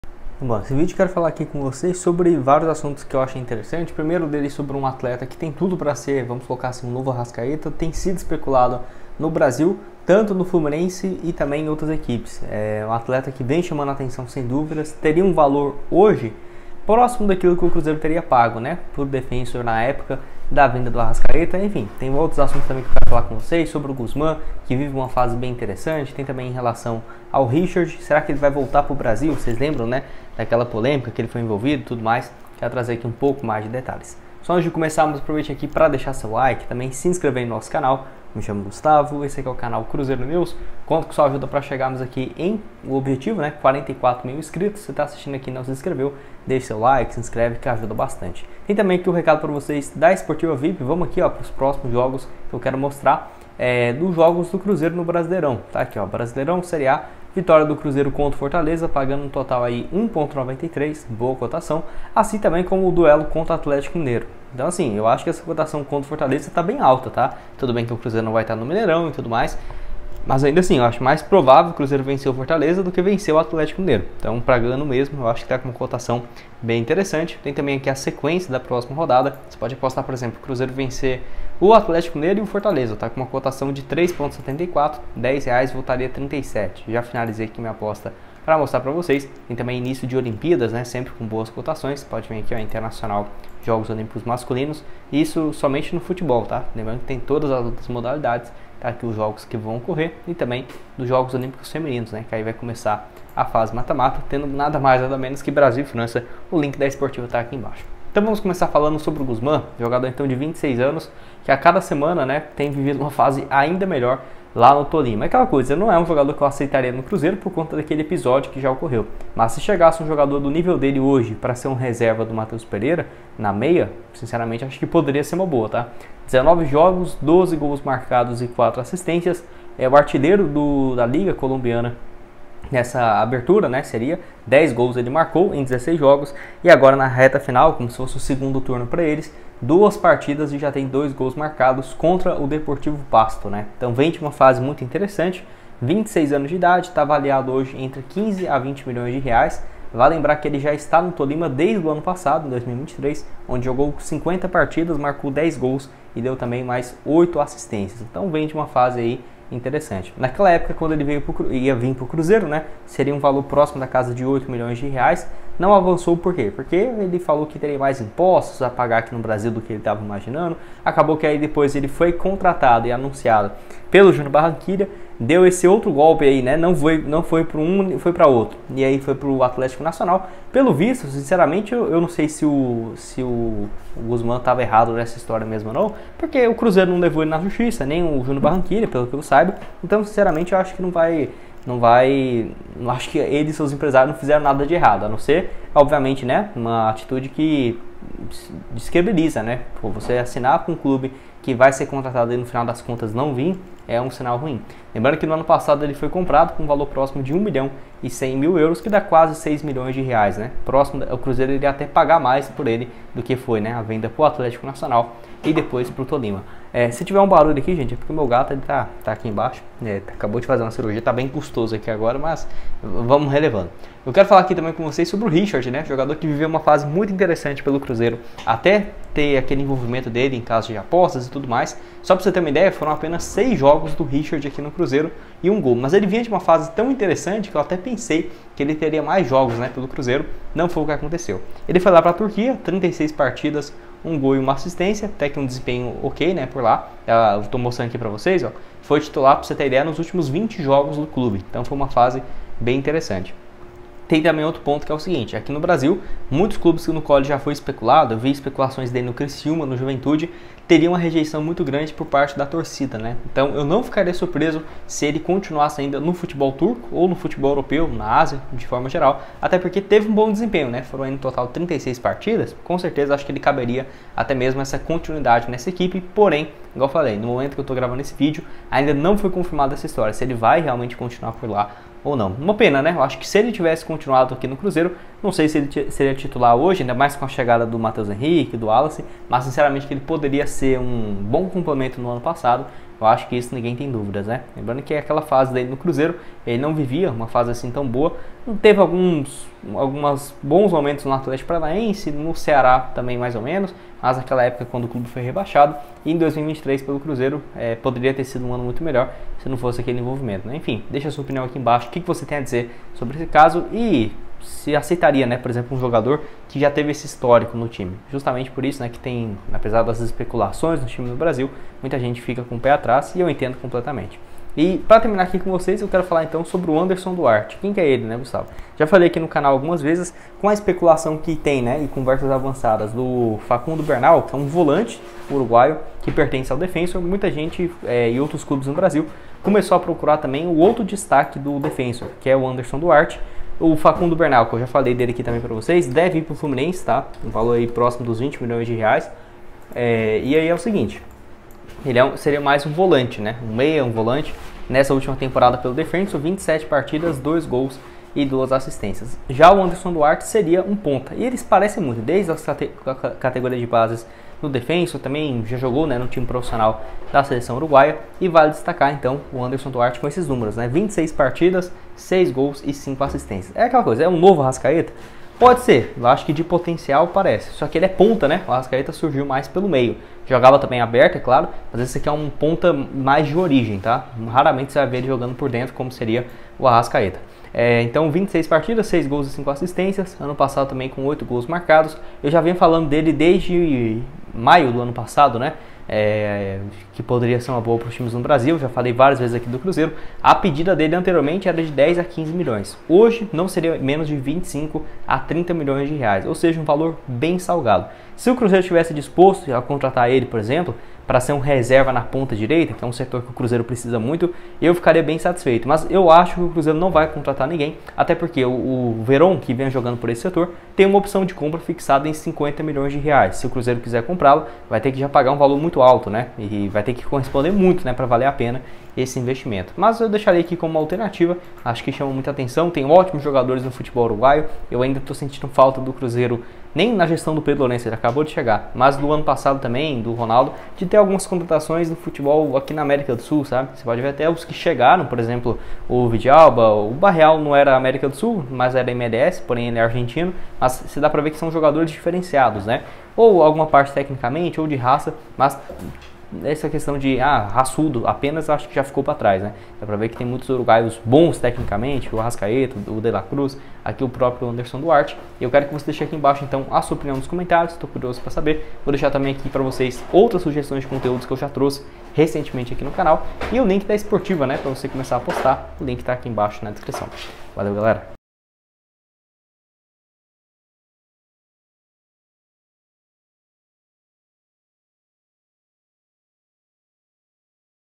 🔥💣GUZMAN VOLTA A TONA NO CRUZEIRO! VAI VIR PRO BR ! RICHARD, NOVO ARRASCA e MAIS
✅💰COMECE NA ESPORTIVA VIP COM 100% DE BÔNUS NO DEPOSITO! A PARTIR DE R$:1 REAL!

🔥Meu perfil no Instagram 👇@gustavo.machado.21 e @cruzeironews21_